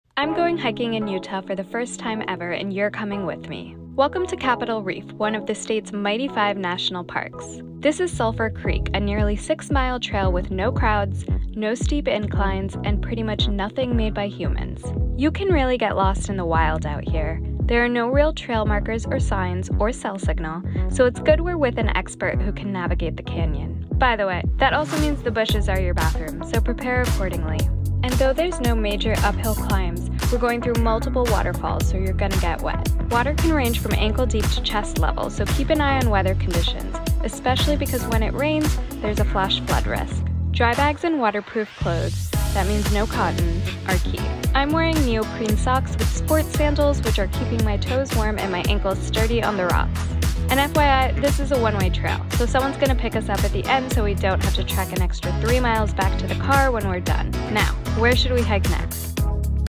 Go on a WATERFALL HIKE in Utah
Utah's Mighty 5 national parks – can you name 'em all? – help you get your steps in. Explore Capitol Reef's red rocks, canyons and waterfalls with LP staffer Deepa, who says this is one of the coolest treks she's ever done because of how secluded and untouched the @Visitutah trail is: "I've gone on hikes where people had to line up to get through, so it was surreal to be pretty much the only group there. It felt very special and peaceful."